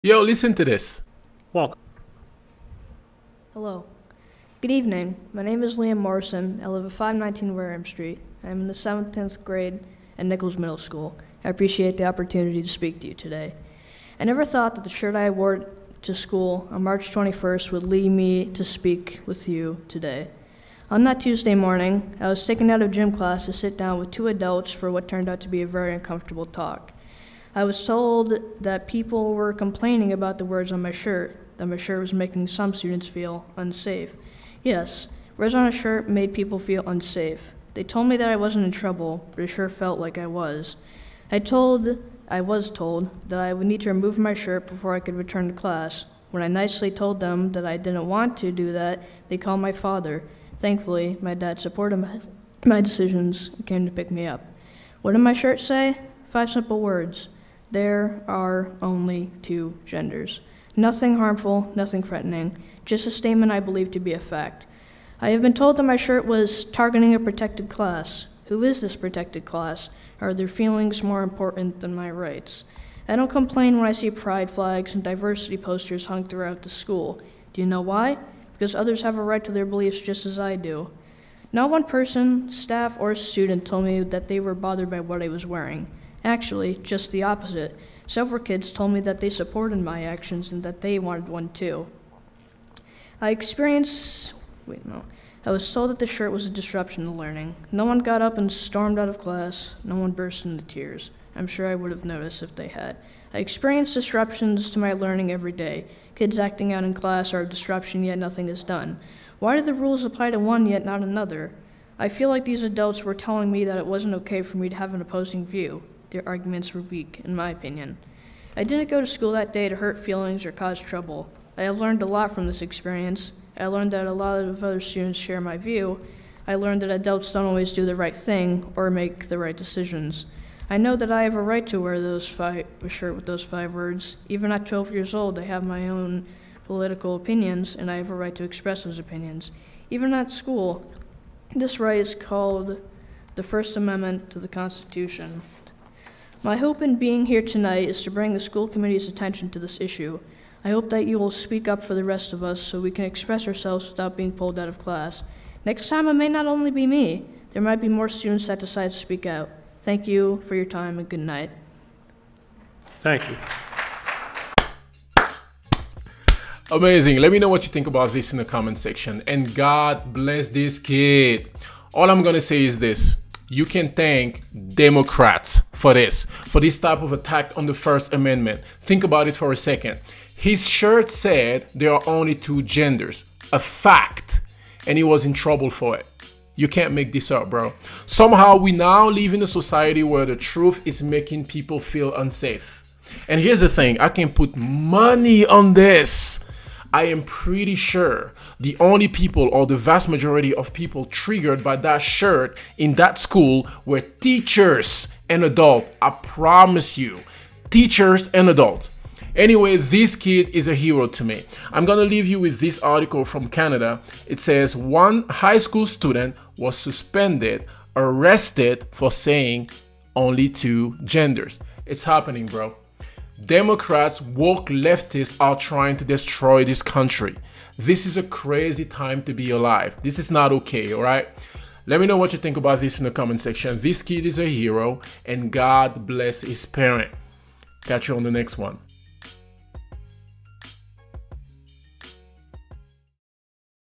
12 Years Old Destroyed School board!! YOU CAN THANK DEMOCRATS FOR THIS
A 12-year-old in @MiddleboroughPS was allegedly sent home from school and told he’s making people feel unsafe for wearing a shirt that said “there are only 2 genders.”